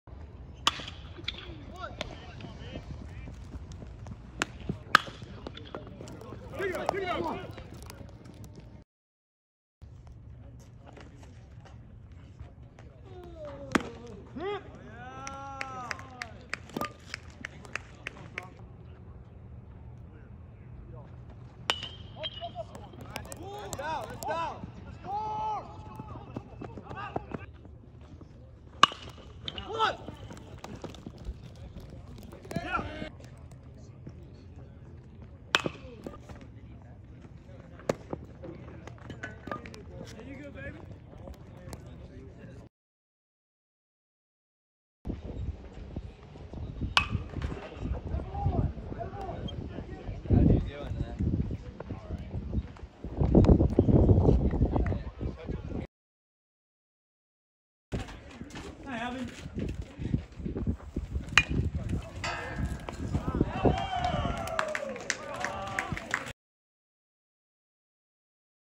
Justice Hallman ‘24 Showcase Series Jones College Mississippi
2 innings 2 hits 1BB 0 runs
10-0 (coach said FB only)

3-5 4bb 1hbp 4R 8-10 ob%

Diving backhand, stand up PO